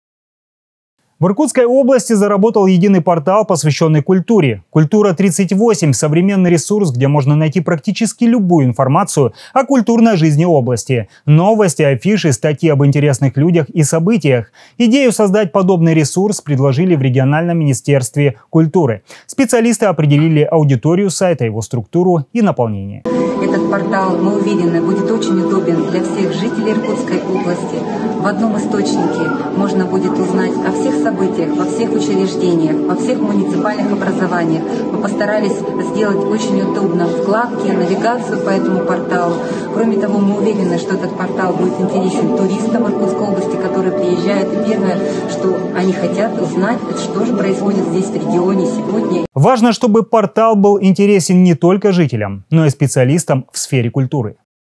Культурный портал
В Приангарье заработал современный интернет ресурс, посвященный культуре